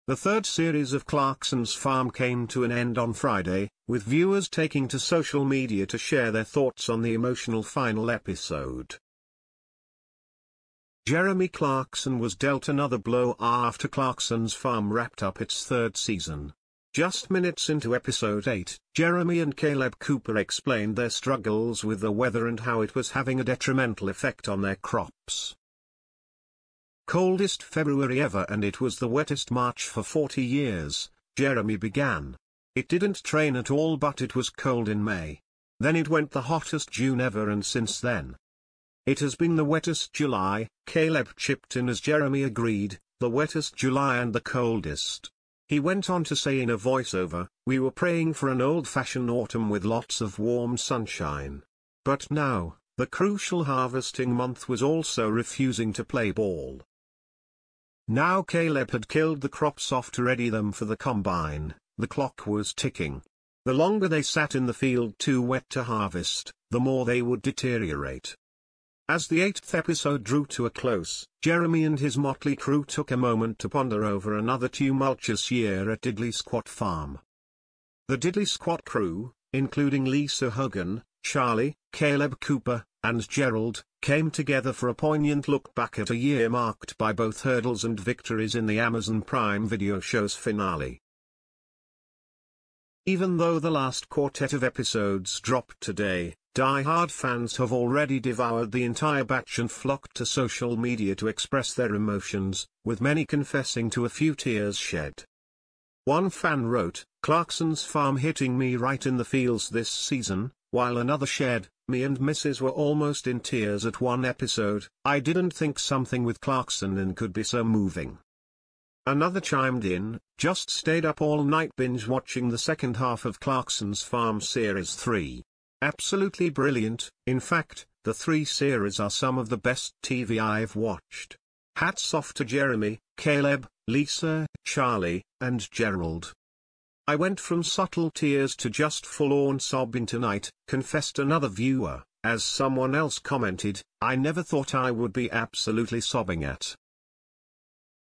Jeremy Clarkson deaIt another bIow on Clarkson's Farm as series comes to an end
Jeremy Clarkson was deaIt another bIow after Clarkson's Farm wrapped up its third season.